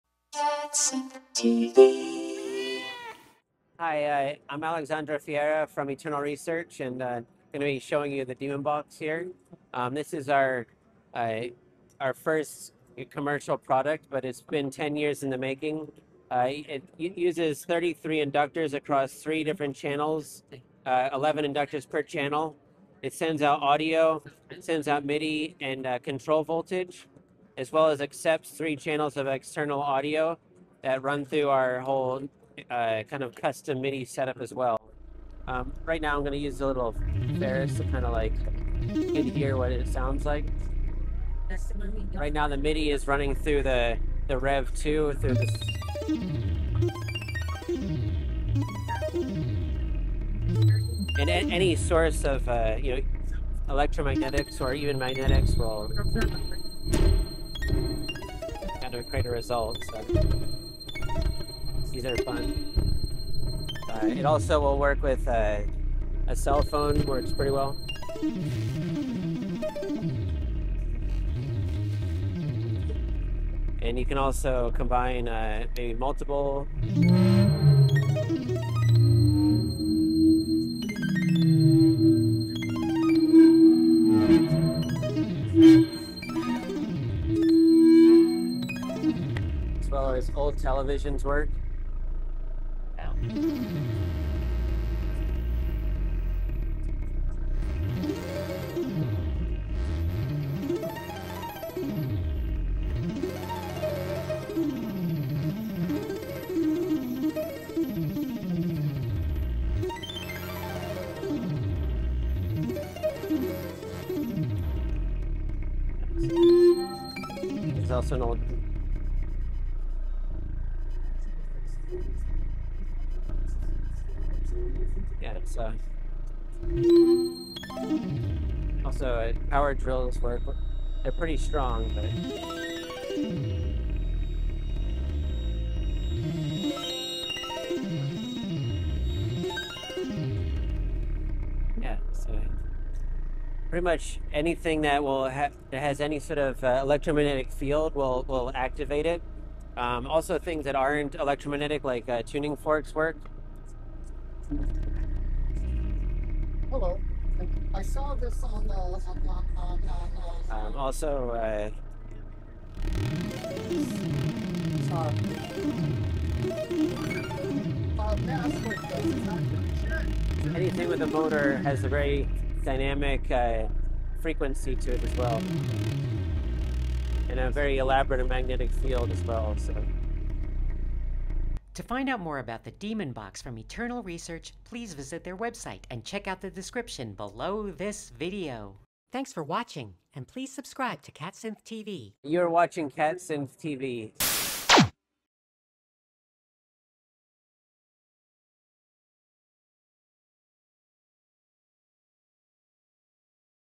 The enigmatic magnetic Demon Box from Eternal Research - Knobcon 2025
Alexandra Fierra of Eternal Research presents the Demon Box, an intriguing instrument based on sensing the dynamic changes in electromagnetic fields.  Any electromagnetic source (e.g., electronic devices, magnets, etc.) or even inert objects will trigger MIDI and CV changes as well as changes in any audio being passed into the instrument.

0:00  Introduction
0:10  The Demon Box
3:01  Conclusion

Please consider supporting this channel to help us bring you more synthesizer tutorials and other content.